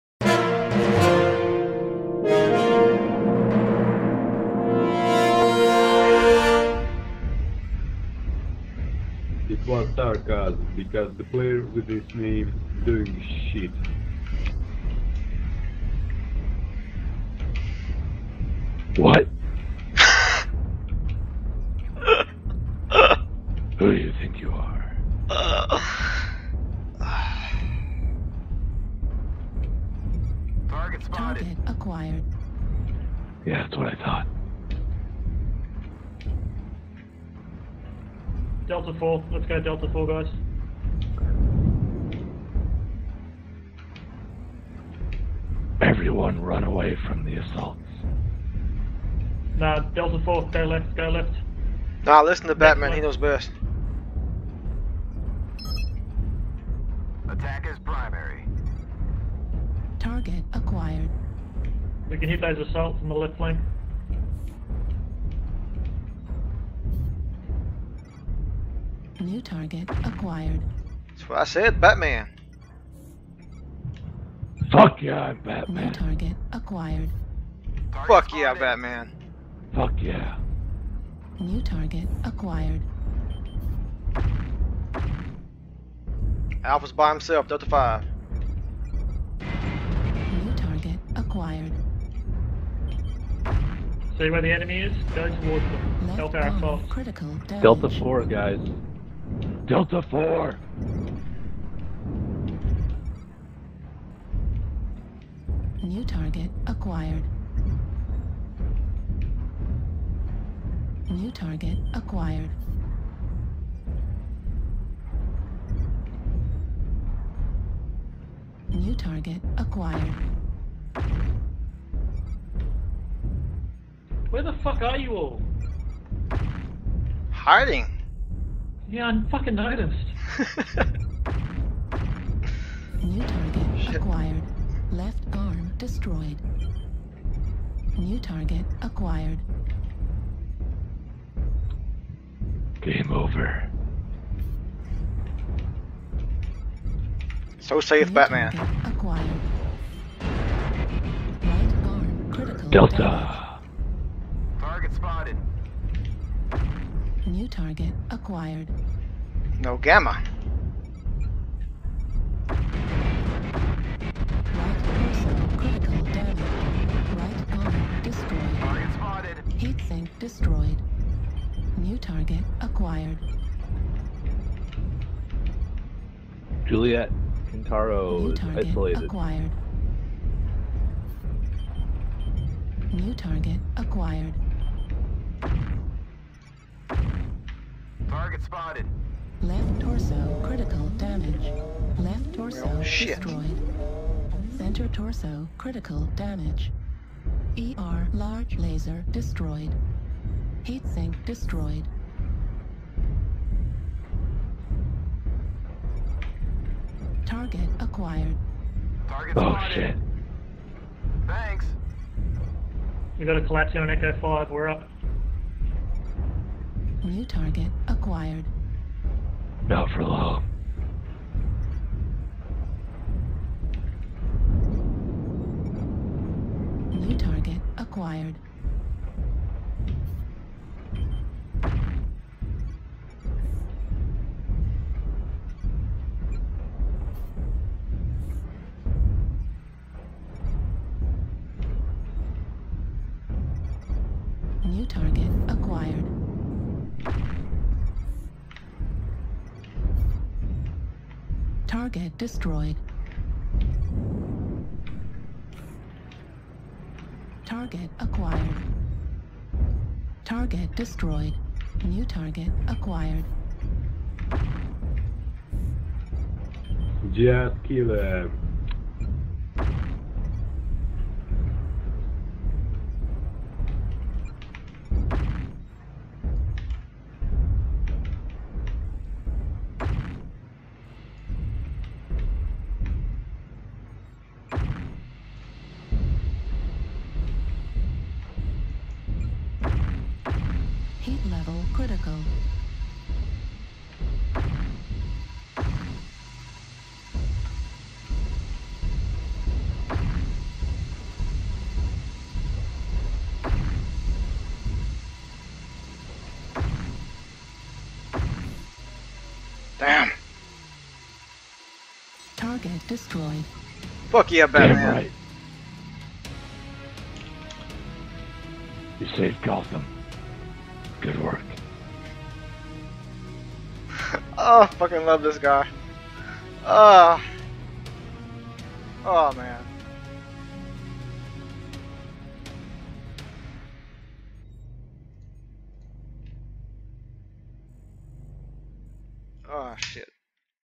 Hunchback HBK-IIC-A "Because Batman"
After a couple games seeing how effective FP builds are in QP, and a couple adult beverages, this happens